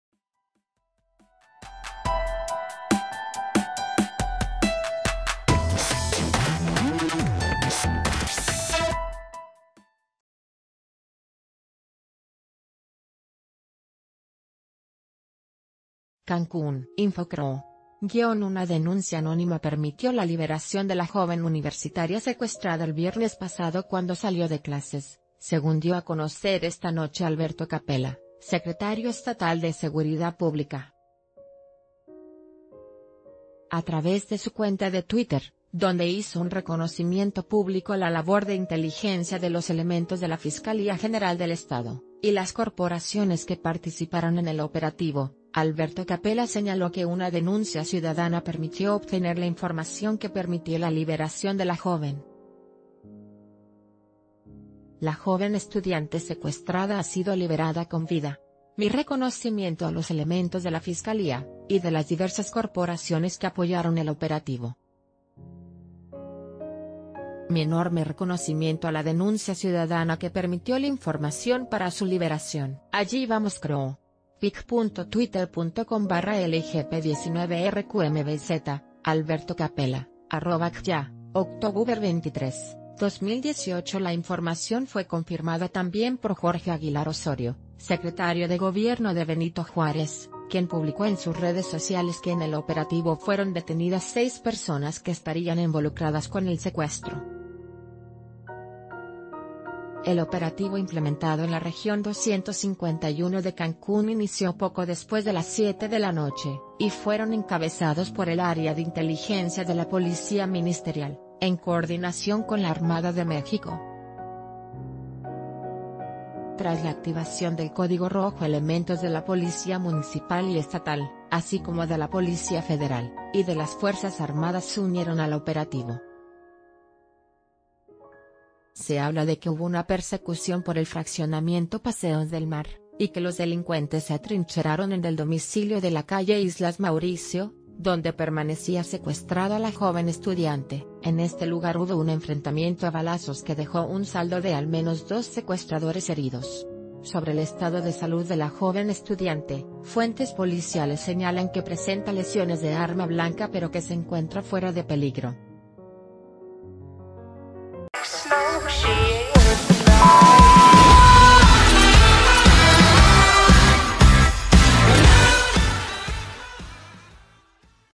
Denuncia anónima permitió rescate de universitaria secuestrada en Cancún
CANCÚN, (Infoqroo).- Una denuncia anónima permitió la liberación de la joven universitaria secuestrada el viernes pasado cuando salió de clases, según dio a conocer esta noche Alberto Capella, secretario estatal de Seguridad Pública. A través de su cuenta de twitter, donde hizo un reconocimiento púb...